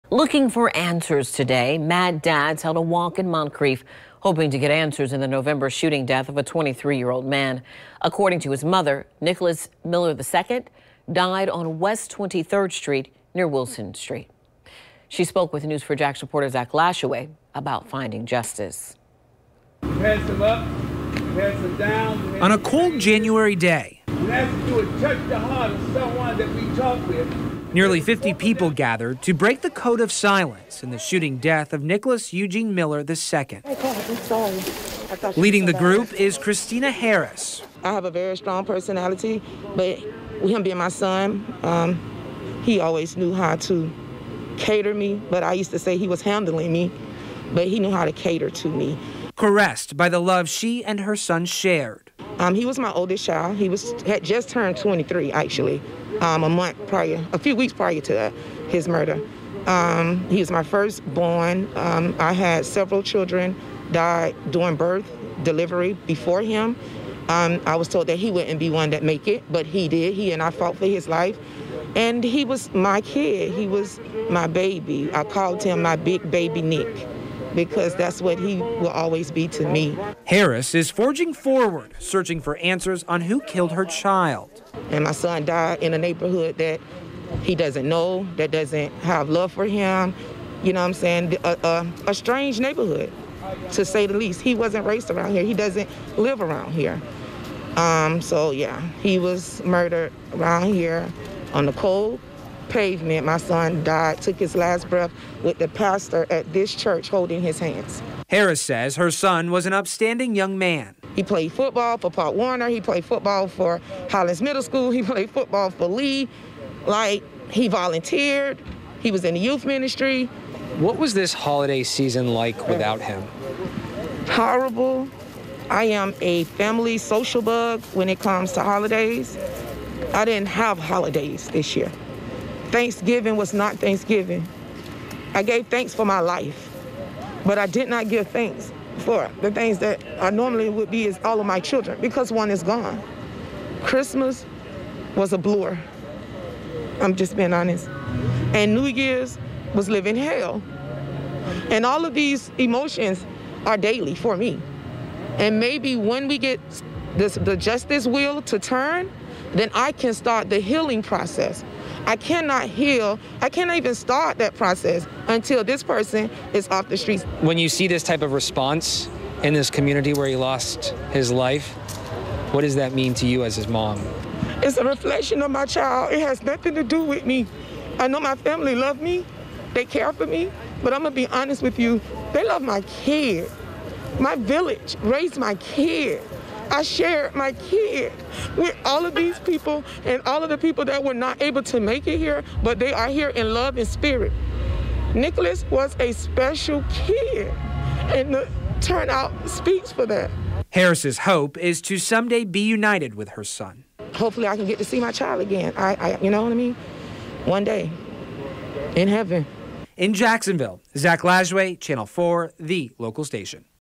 Moncrief walk for answers in unsolved shooting death
MAD DADS held a walk in Moncrief on Saturday, hoping to get answers in the November shooting death of a 23-year-old man.